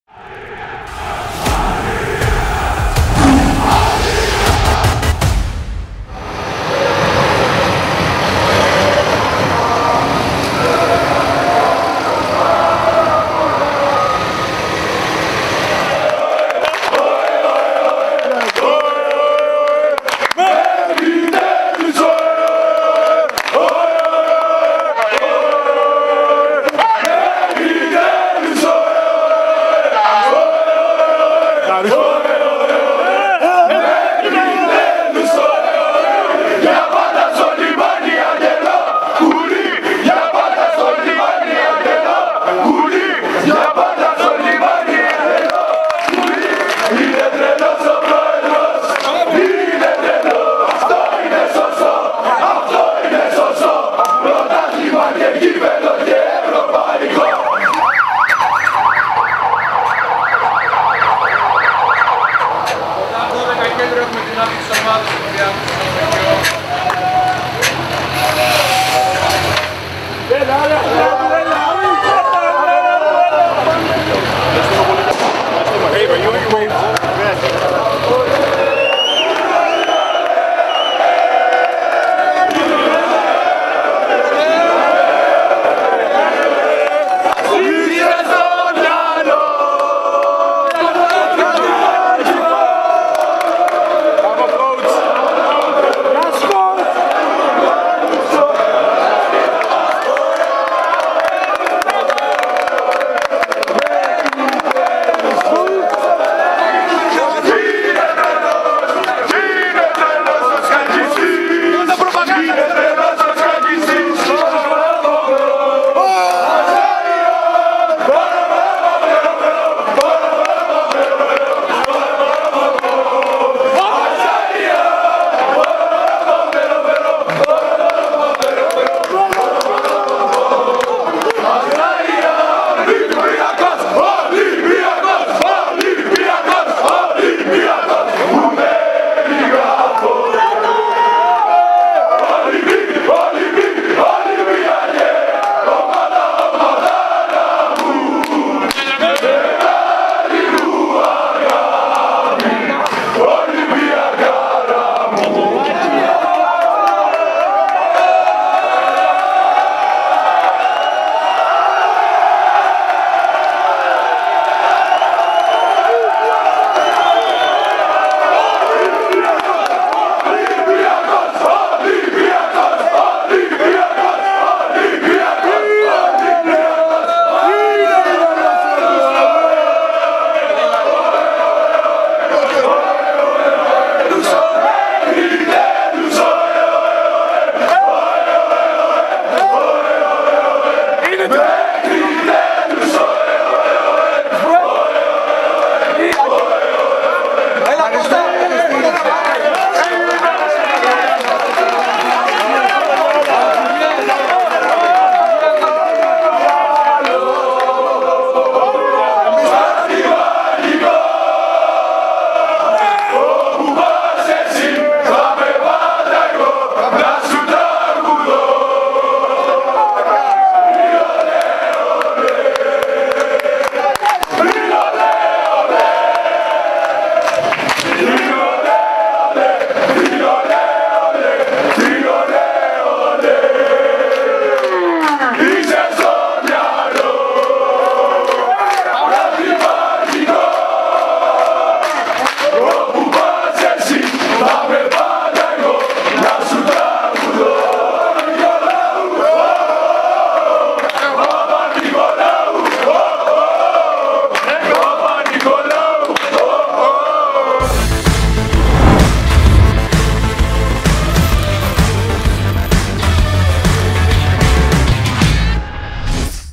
Αποθεώθηκαν οι Πρόεδροι και η αποστολή
Αποθεώθηκαν οι Πρόεδροι της ΚΑΕ Ολυμπιακός, Παναγιώτης και Γιώργος Αγγελόπουλος και η αποστολή των "ερυθρολεύκων" κατά την επιστροφή τους στο ξενοδοχείο μετά την κατάκτηση του Κυπέλλου Ελλάδος.